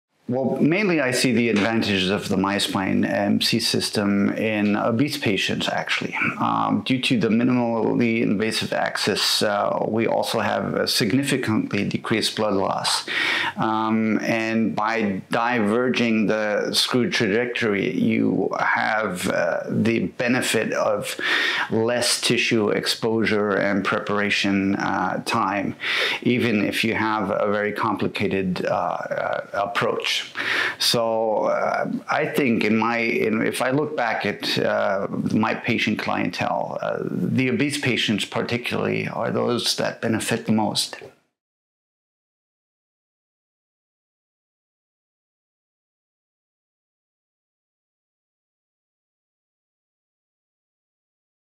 MySpine MC Experience - PD Dr. Med. Habil Sven Kevin Tschöeke
Hear PD Dr. Med. Habil Sven Kevin Tschöeke talking about the core advantages of MIS MySpine MC Technology, part of Medacta’s MySolutions Personalized Ecosystem.

MySpine MC Personalized MIS Solution is a 3D printed patient-matched solution, which can help position vertebral screws in the midline cortical approach. It provides high accuracy in screw positioning and robust posterior fixation through a minimally invasive, muscle-sparing surgery.